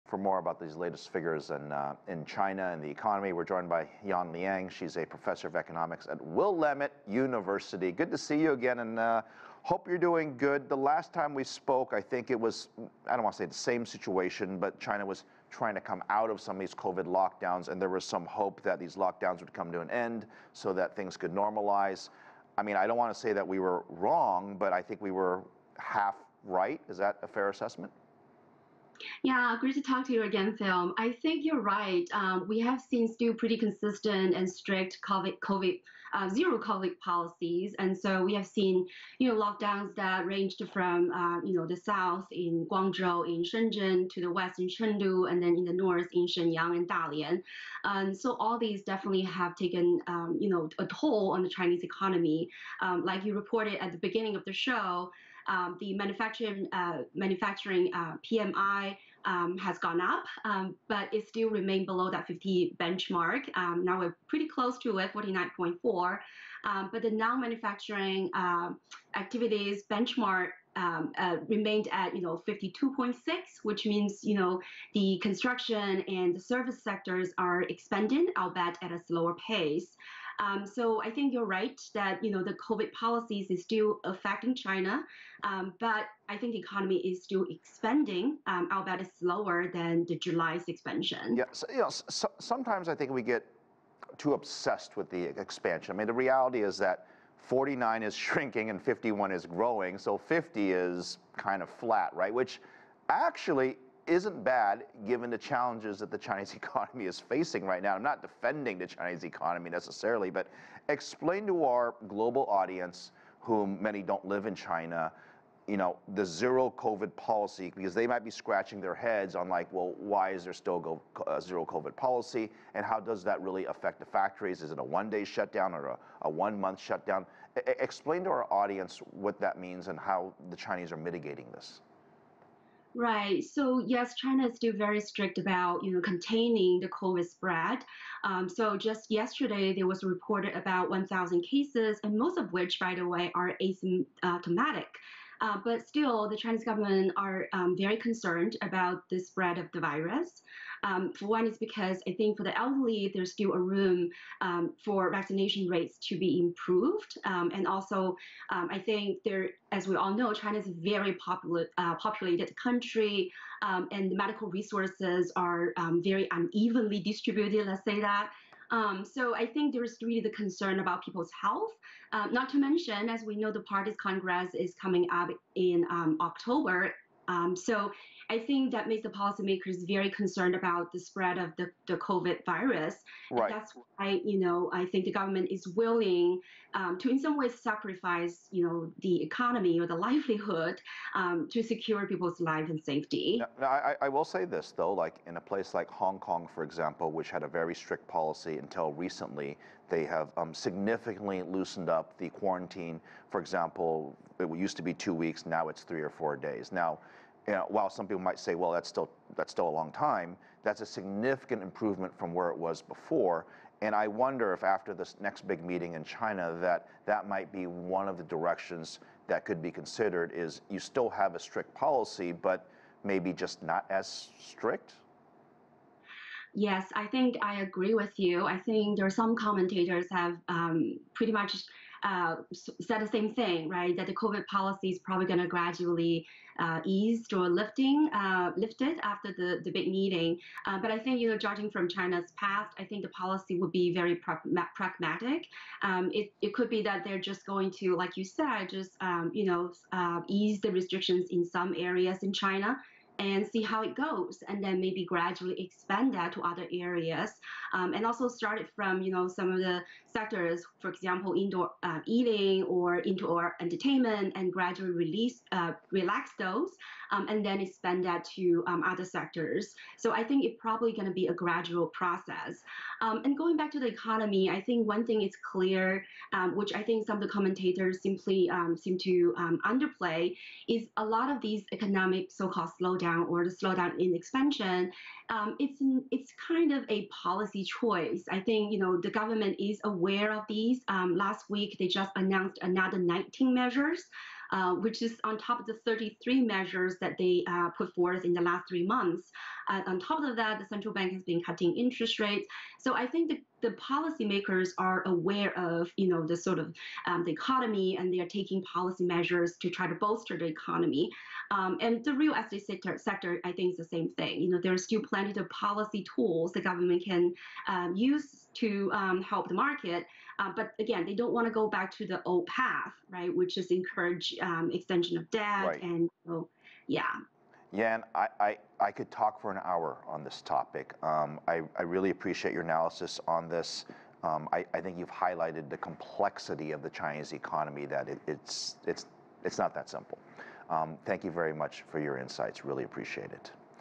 Yan Liang talks about the latest figures on China's economy
CGTN's Phillip Yin spoke with Yan Liang, professor of economics at Willamette University, about the latest figures on China's economy.